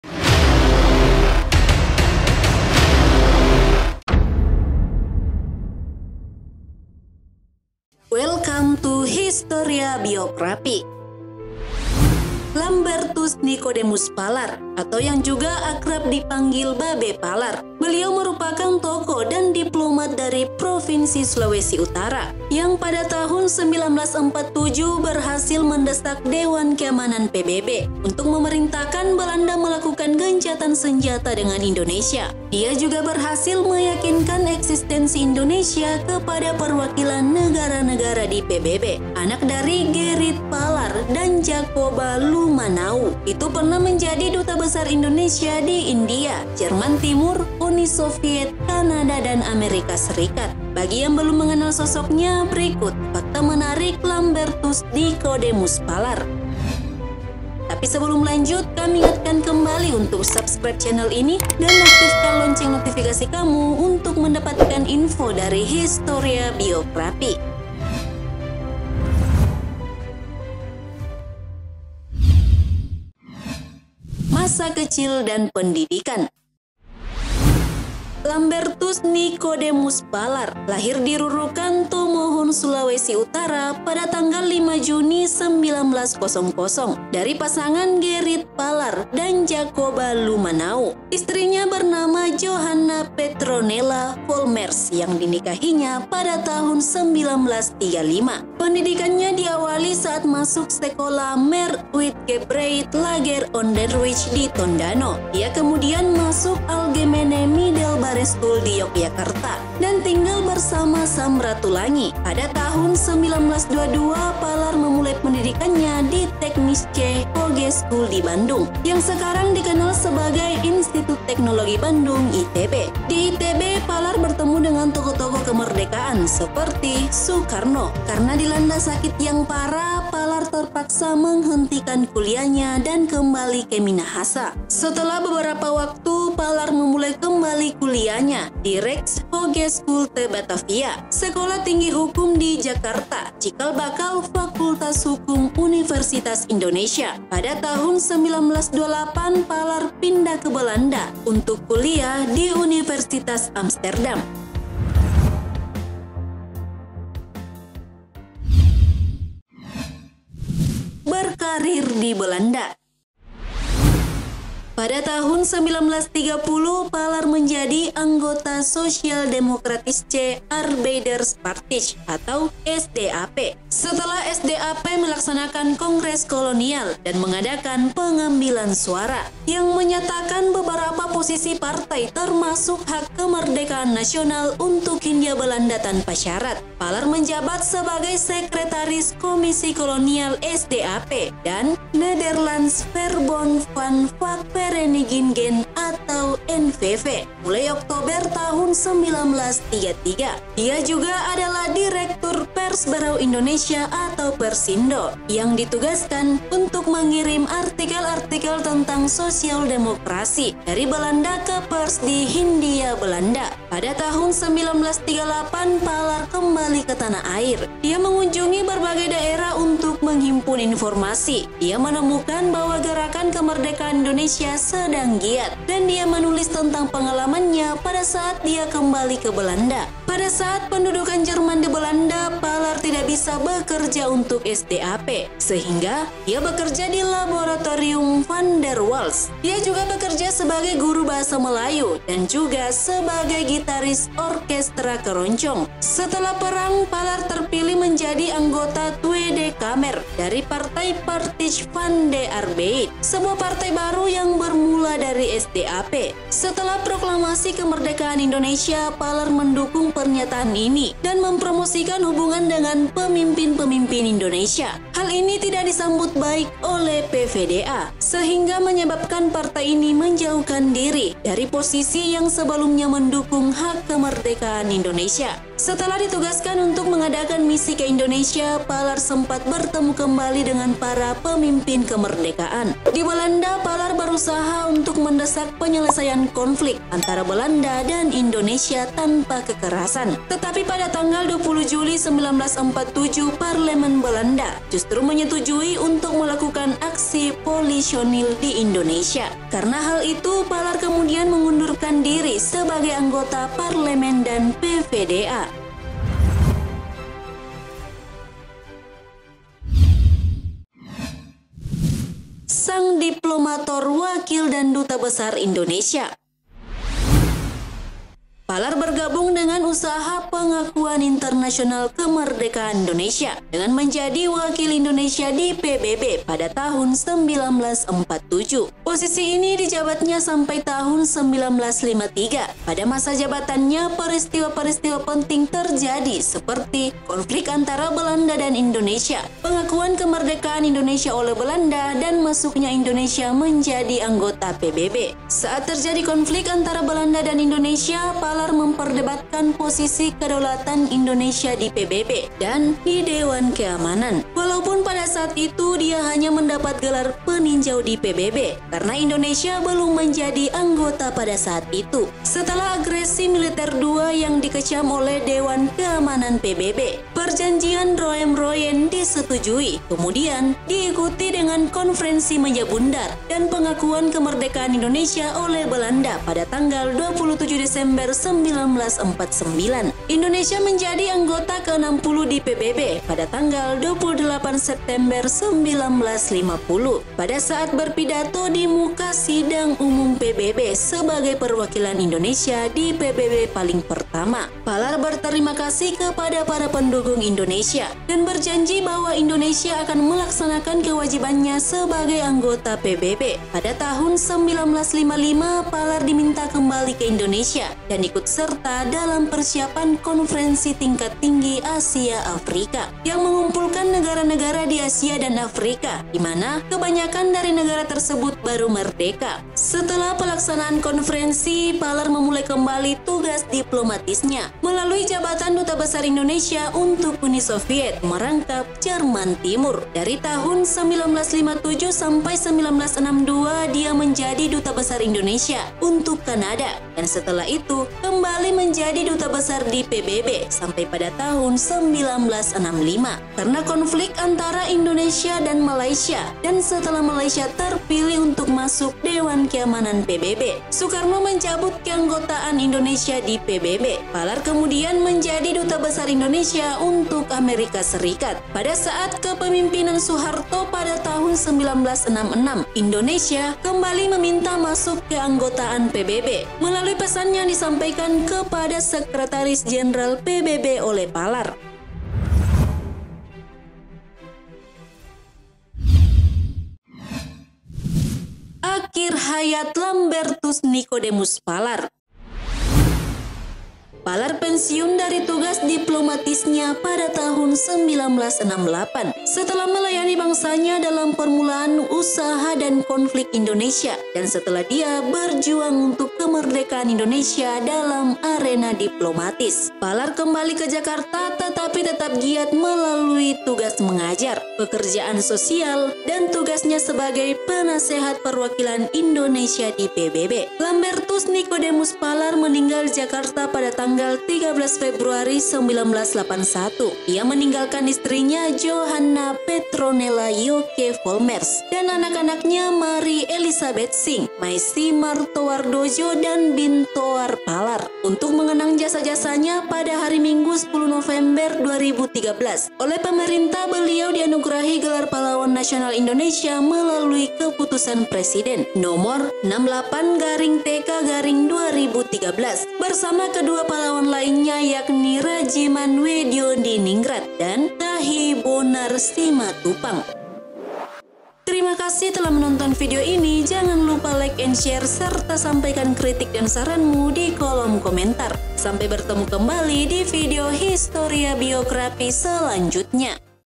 Sosok Penting dibalik Pengakuan PBB dan Dunia Internasional kepada Indonesia
Lambertus Nicodemus Palar atau yang juga akrab dipanggil Babe Palar tersbeut merupakan tokoh dan diplomat dari Provinsi Sulawesi Utara yang pada 1947 berhasil mendesak Dewan Keamanan PBB untuk memerintahkan Belanda melakukan gencatan senjata dengan Indonesia. Dia juga berhasil menyakinkan eksistensi Indonesia kepada perwakilan negara-negara di PBB.
Anak dari Gerrit Palar dan Jacoba Lumanauw itu pernah menjadi Duta Besar Indonesia di India, Jerman Timur, Uni Soviet, Kanada, dan Amerika Serikat.